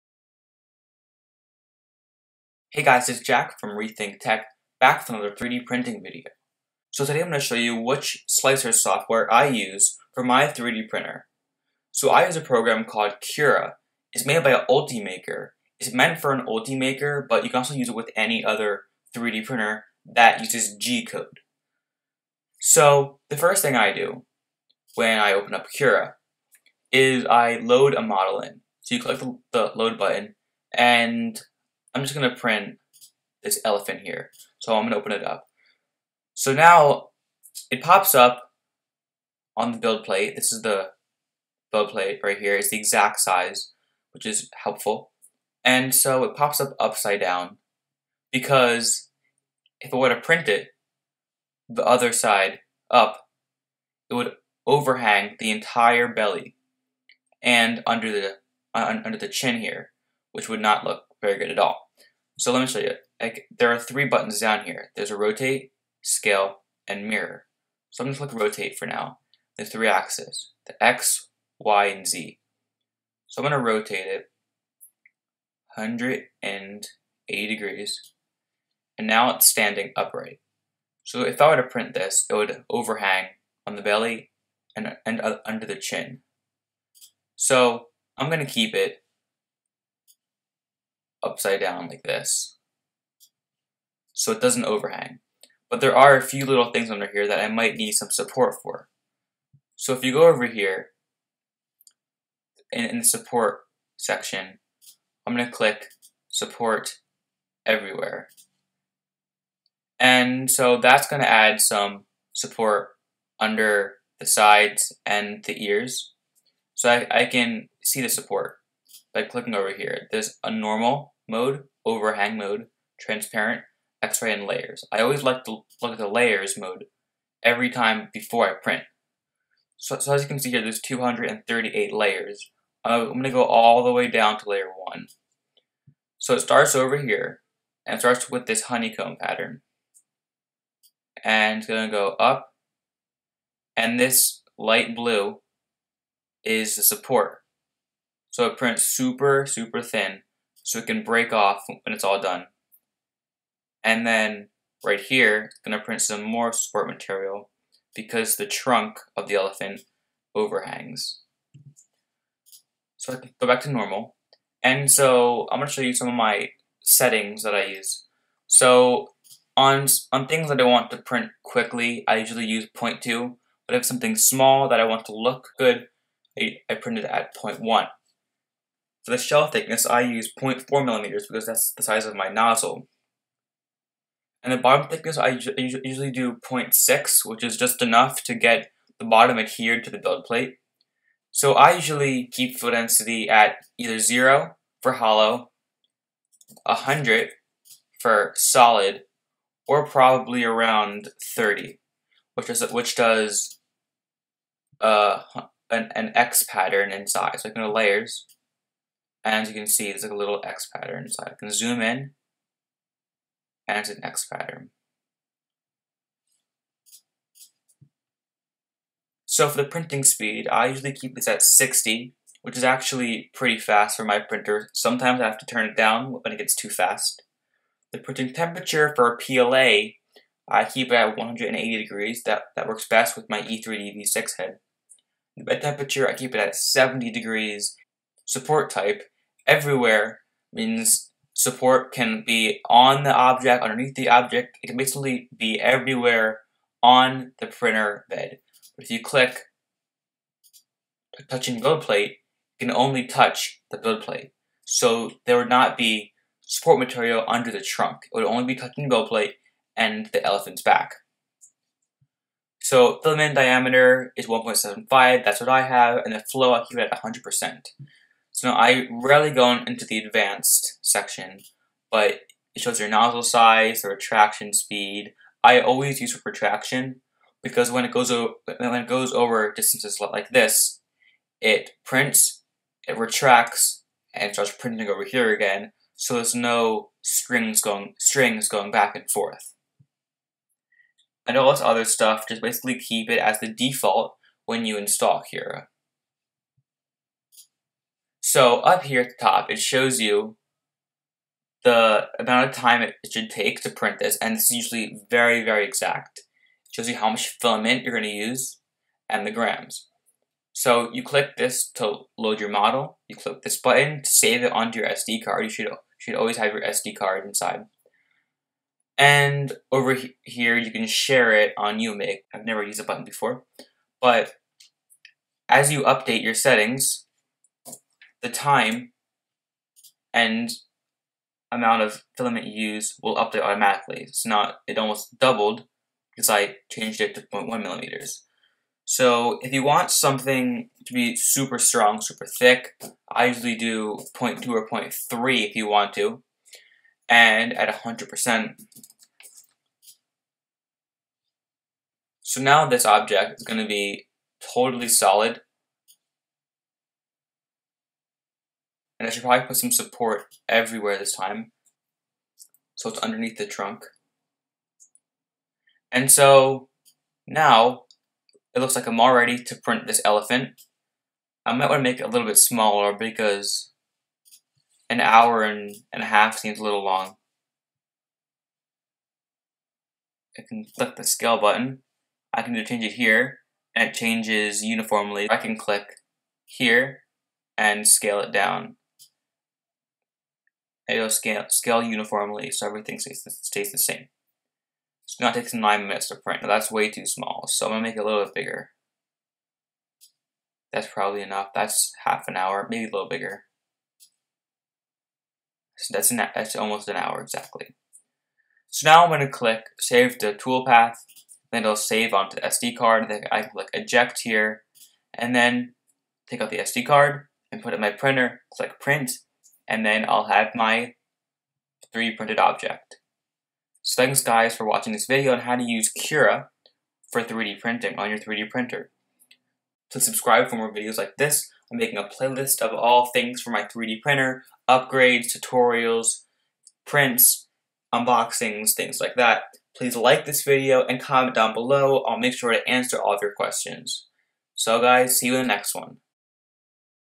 Sunhokey reprap Prusa i3 3D Printer: Cura slicer software
I show which slicer I use for my printer and show how to use it correctly. Also, I show the settings I use to print.